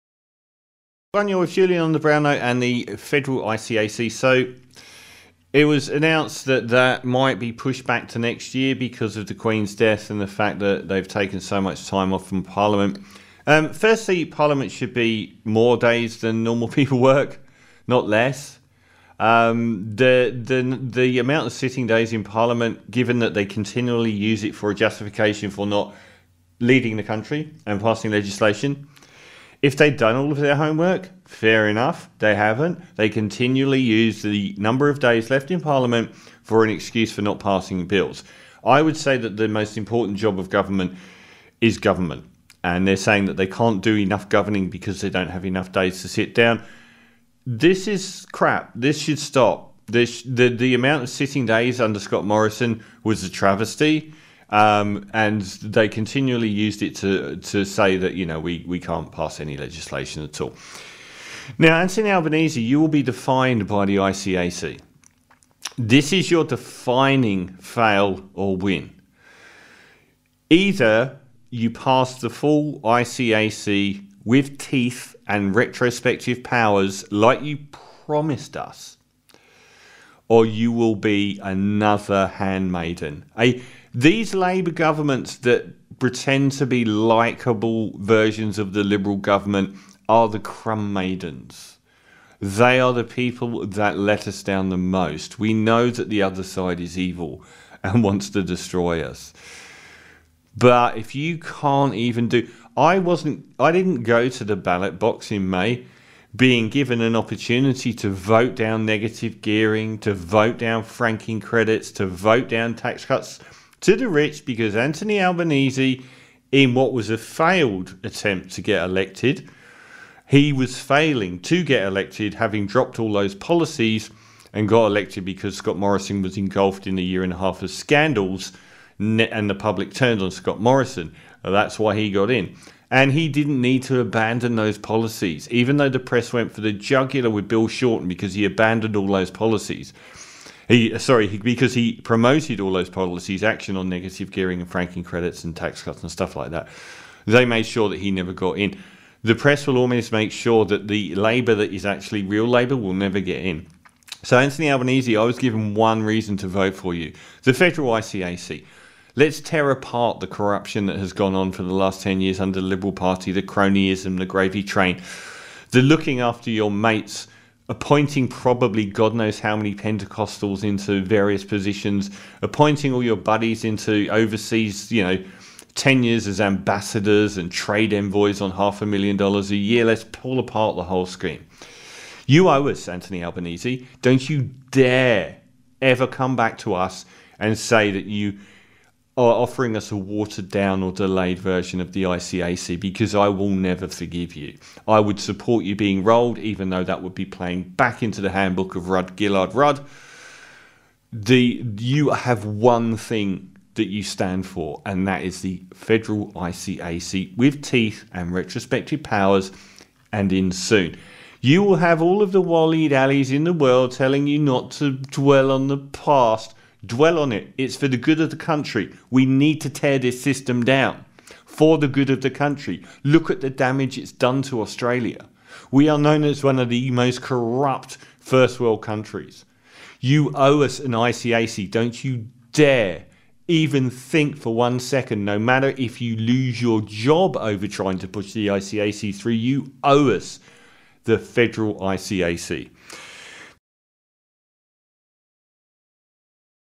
Anthony Albanese And The Curse Of The Fedral ICAC
Having deprived me being able to vote to remove franking credits, negative gearing and tax cuts for the rich (all essential Labor policies the new Australian prime minister abandoned for zero votes...) I had one solid, rolled gold policy to vote on - a federal ICAC, with teeth and retrospective powers, to tear down arguably the western world's most corrupt governance. This promise has now been punted down the road, due to the Queen's death and Albanese has so far a history of avoiding strong or controversial policies. You had better not let us down even 1%, the federal ICAC is your legacy.